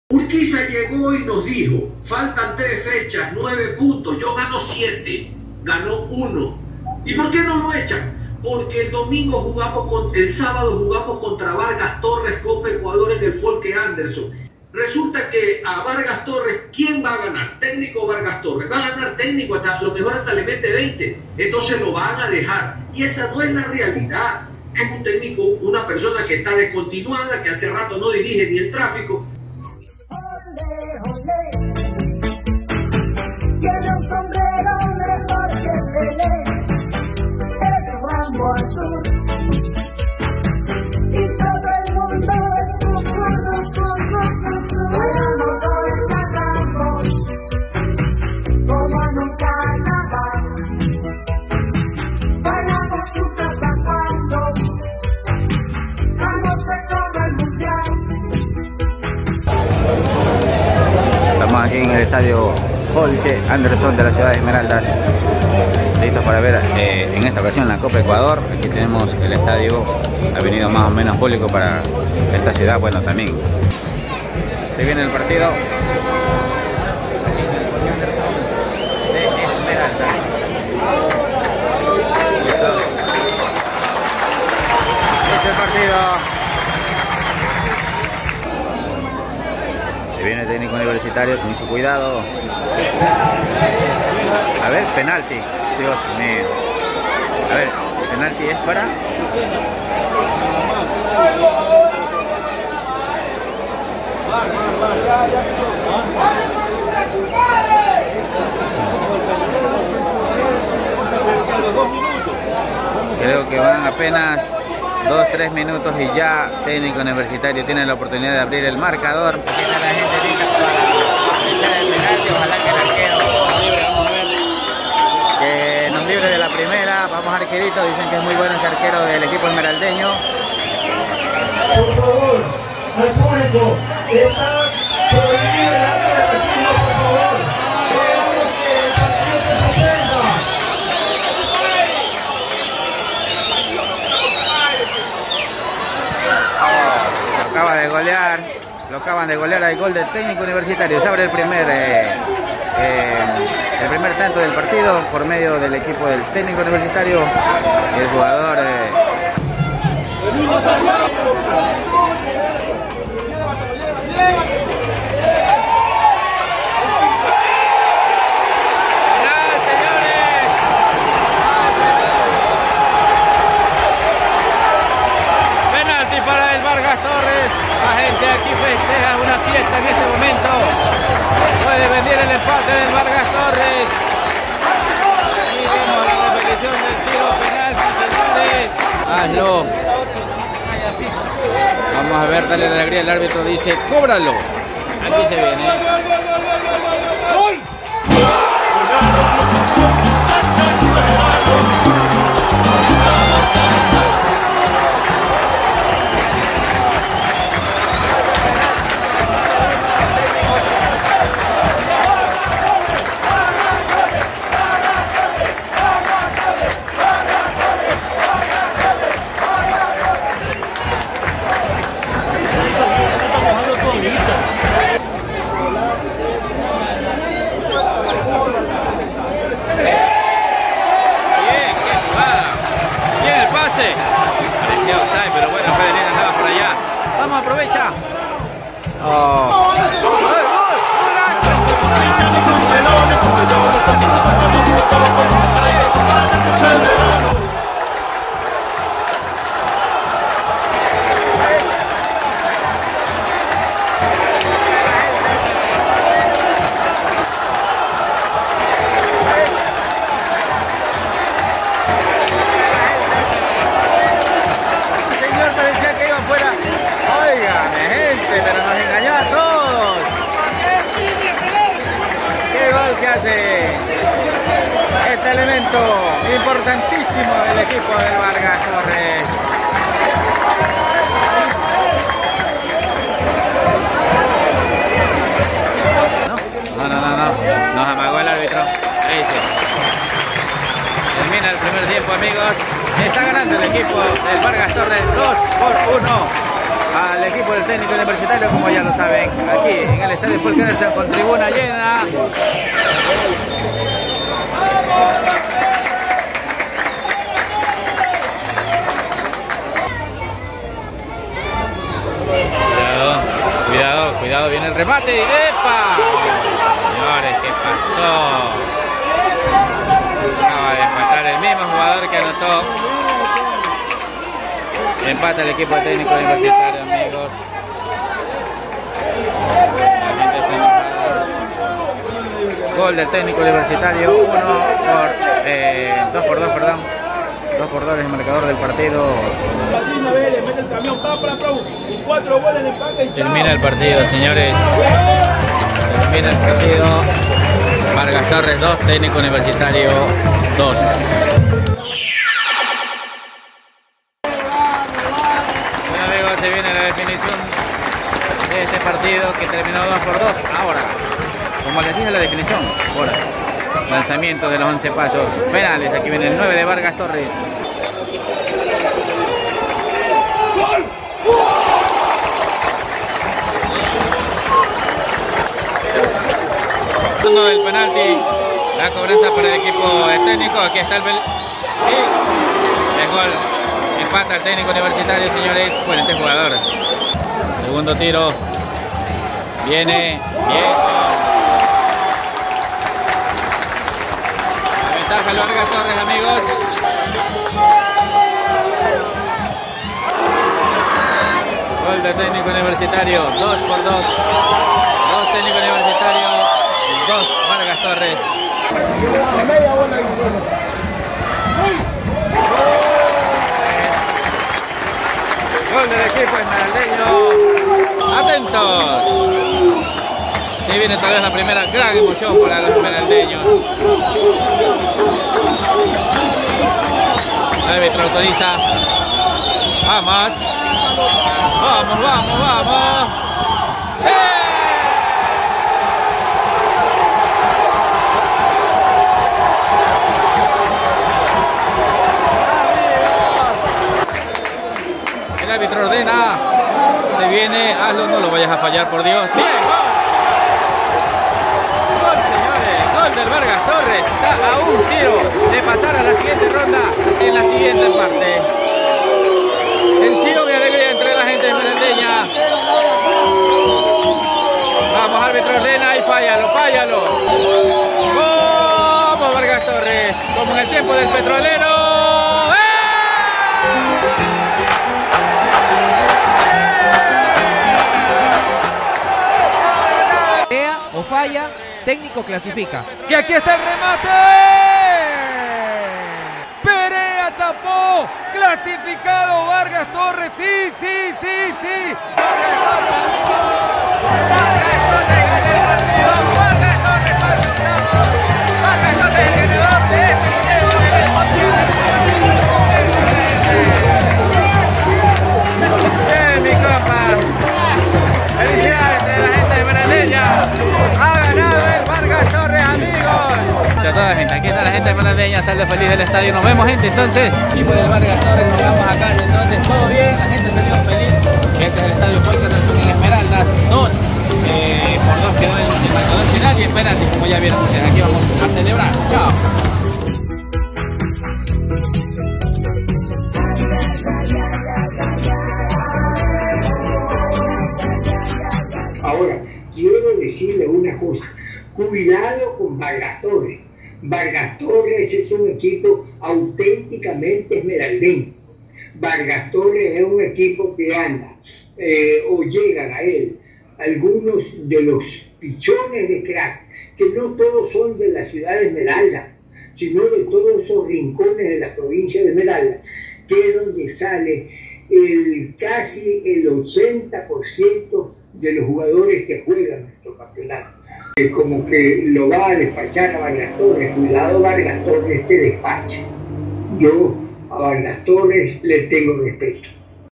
TECNICO UNIVERITARIO ELIMINADO A MANOS DE VARGAS TORRES COPA ECUADOR (Estadio Folke Anderson/Esmer)
VARGAS TORRES CLUB ESMERALDEÑO DEJA FUERA A TECNICO UNIVERSITARIO DE AMBATO EN PARTIDO UNICO JUGADO EN EL ESTADIO FOLKE ANDERSON DE ESMERALDAS, LUEGO DE EMPATAR A 2 EN EL TRÁMITE DEL COTEJO, Y YA, EN DEFINICION DE TIROS PENALES EL EQUIPO AMBATEÑO ERRÓ EN DOS OCASIONES PROVOCANDO CON ELLO SU ELIMINACION Y PASE DE LOS ESMERALDEÑOS A LOS OCTAVOS DE FINAL DE LA COPA ECUADOR, CON LA POSIBILIDAD MEDIATA DE ENCONTRARSE CON LA U. CATOLICA DE LA CAPITAL.